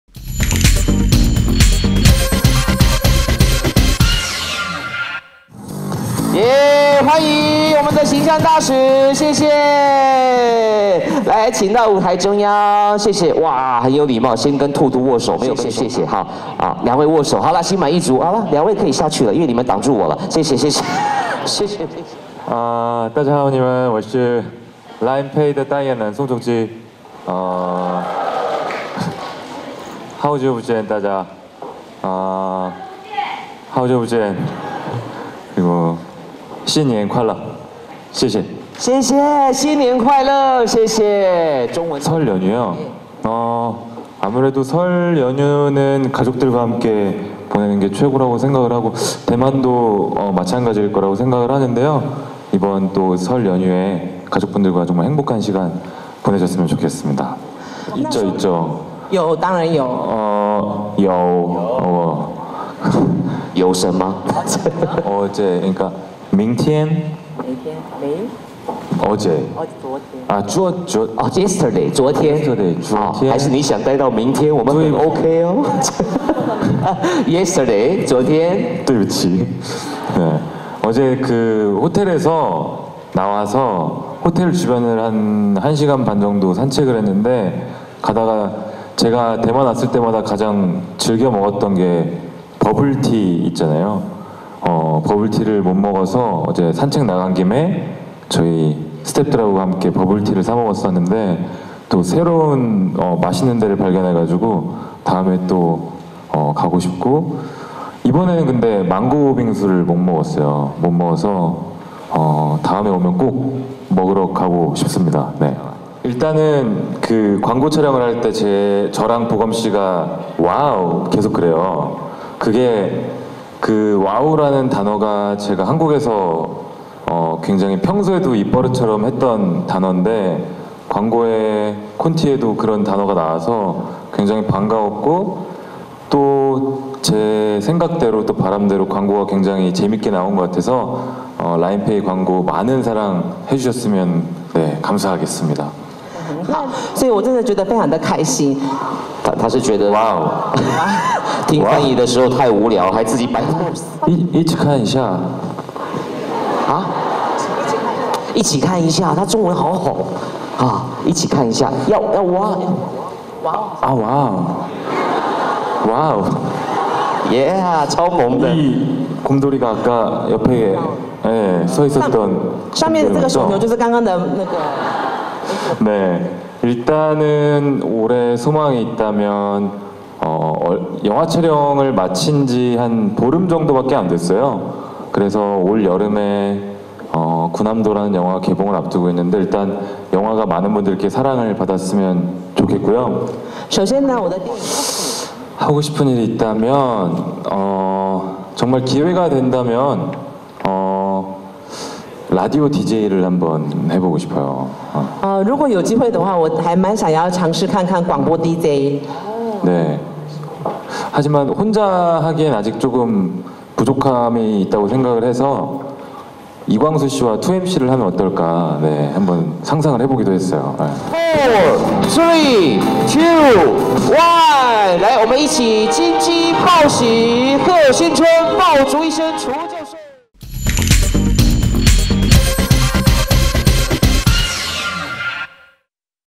[S영상] 송중기, "기회가 된다면 이광수와 함께 라디오DJ 해보고싶다"(대만 LINE Pay)
[스타데일리뉴스=소가영 기자] 송중기가 지난 23일, 대만에서 열린 'LINE Pay' 행사에 참석했다.